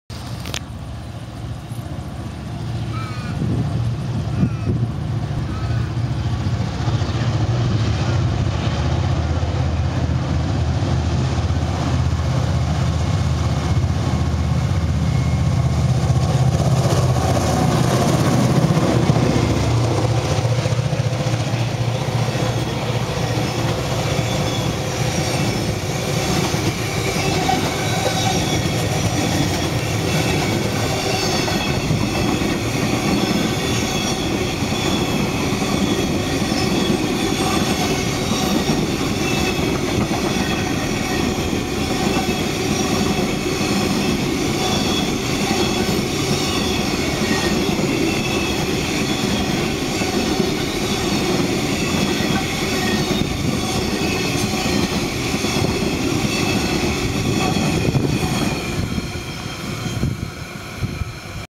ARG freight train passing through Waroona
Australian Rail Road Group train passing through Waroona in western Australia on the south west main